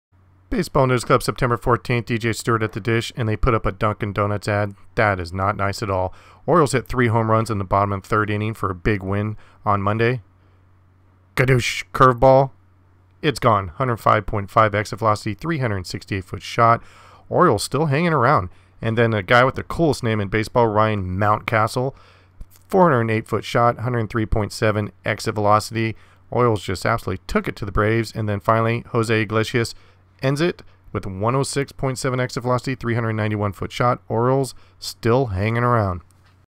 Baltimore Orioles (3) HR's in one inning 9.14
** Baltimore just keeps hanging around and really put it to the Atlanta Braves scoring (14) runs. DJ Stewart, Ryan Mountcastle and Jose Iglesias hit HR's in the same inning.